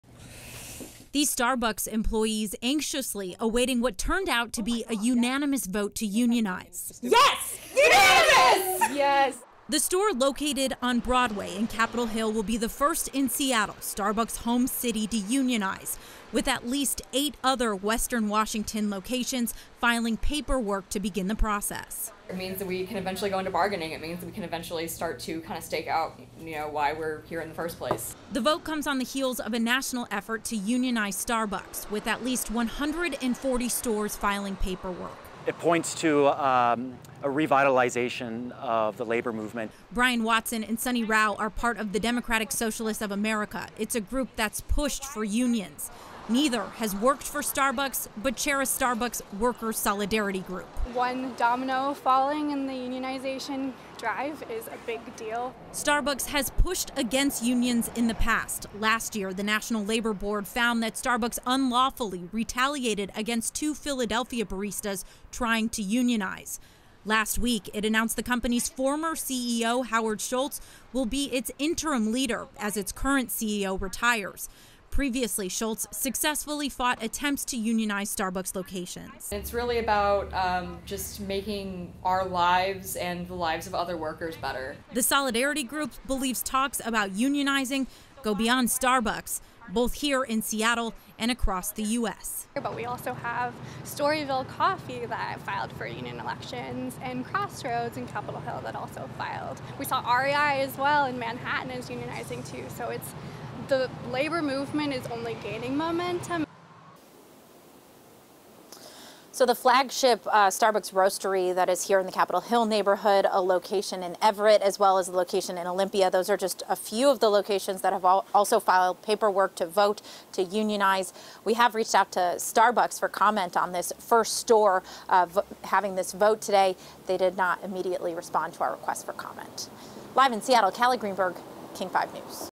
First Seattle Starbucks workers join union
The move makes the Starbucks store the seventh to unionize in the country.